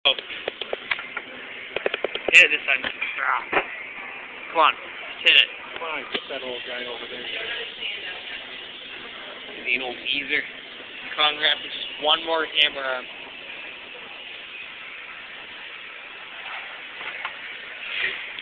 Gramps does a hammer arm
In Boston Pizza gramps does a hammer arm